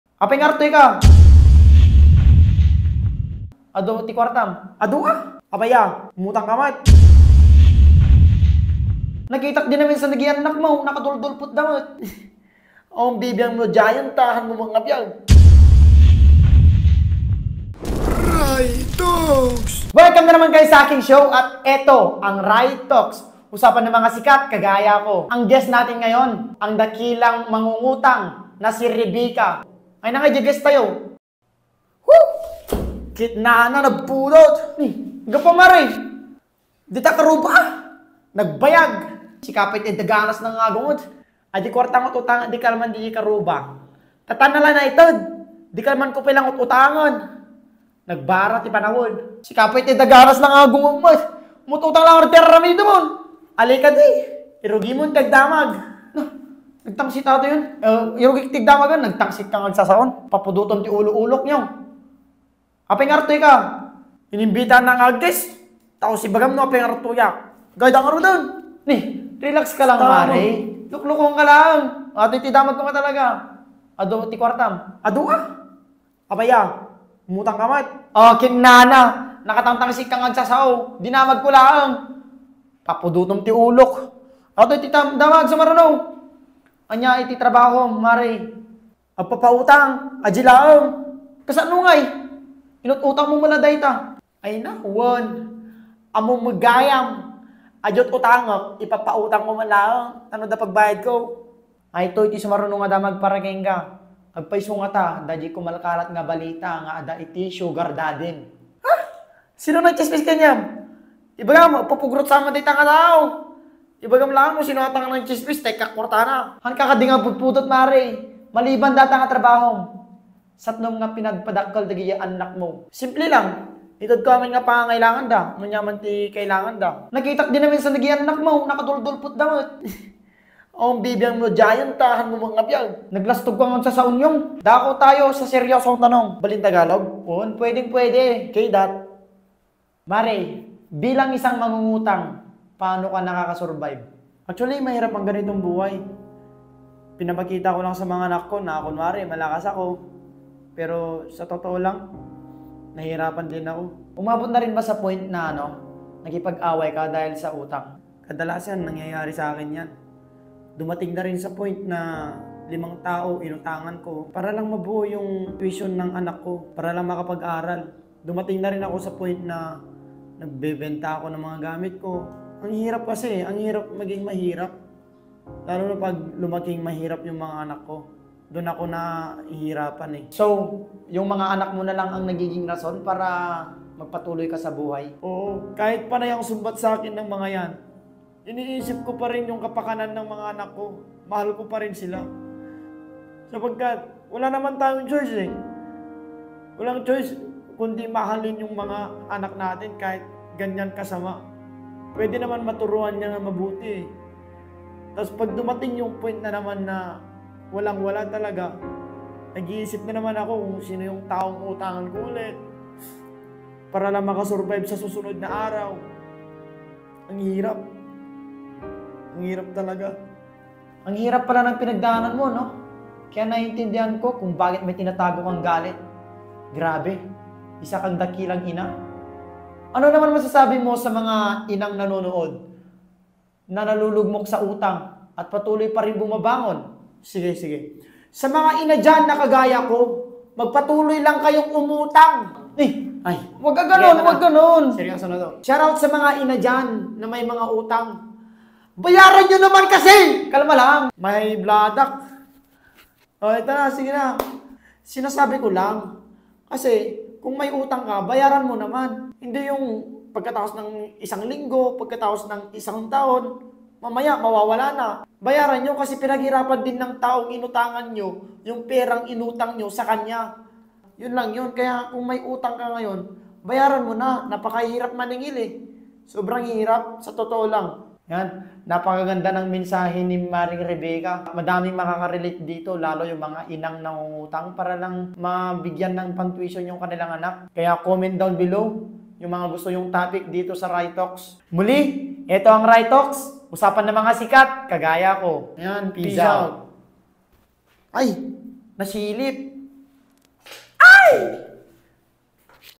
Ribika Interview - RyeTox
Interview kay Ribika, ang dakilang mangungutang may hirap na dinadamdam!